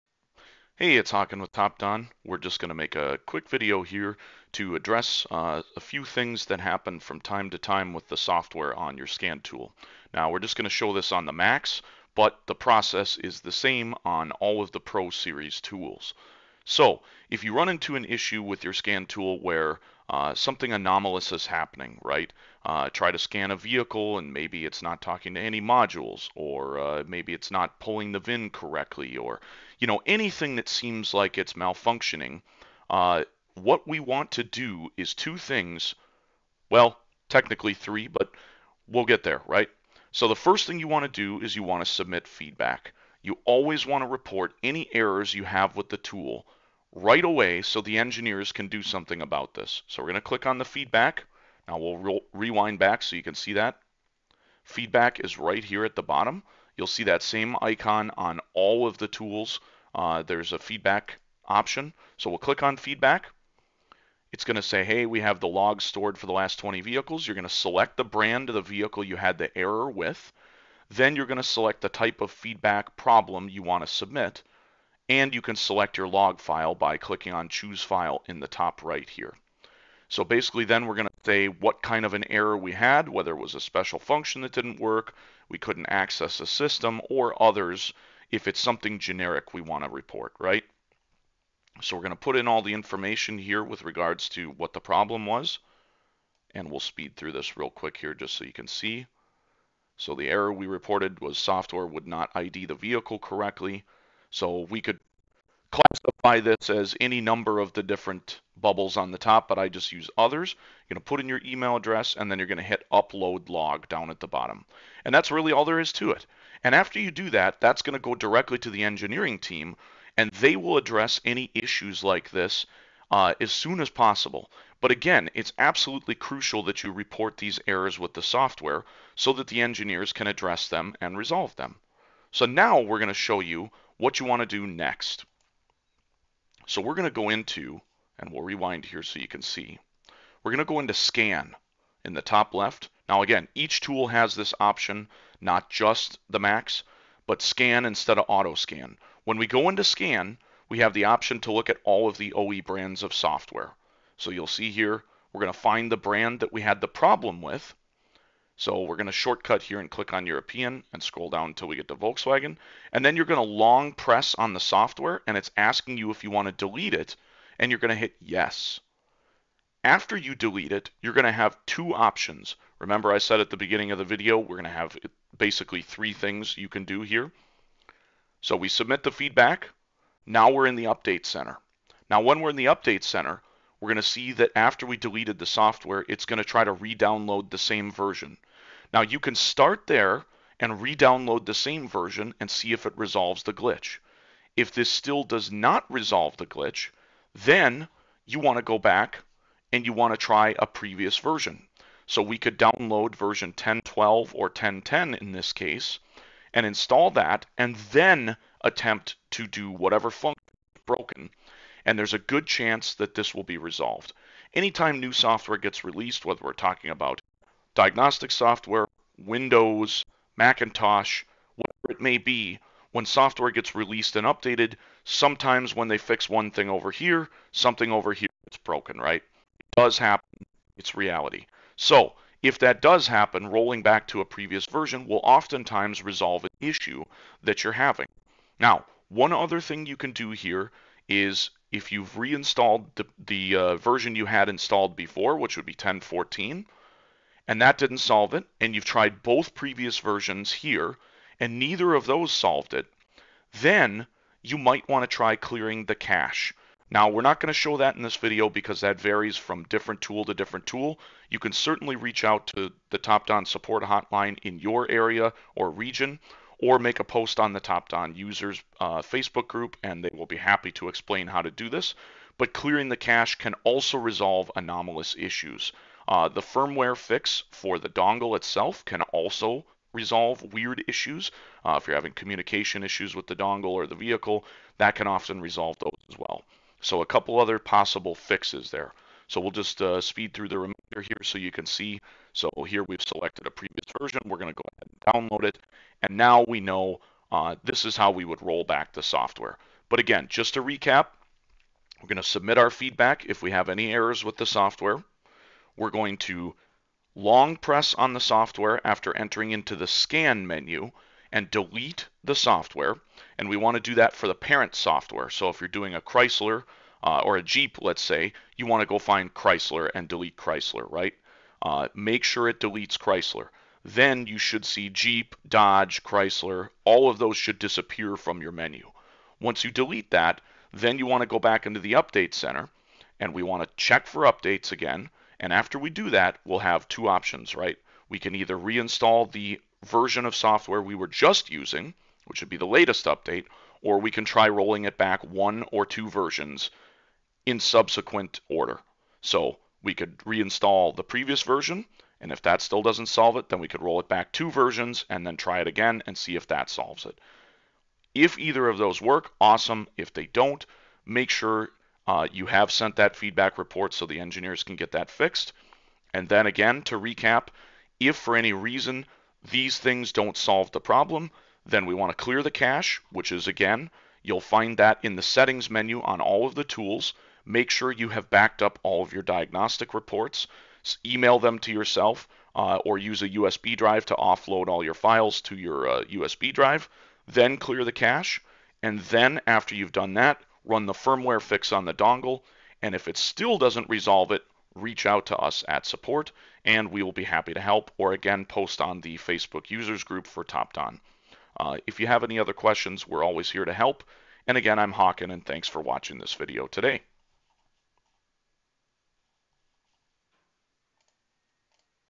Fixing Software Issues on TOPDON Phoenix Series
Having issues or anomalies with your TOPDON Phoenix scan tool? In this edition of Tech Tips, Haakan explains how to potentially resolve the issue.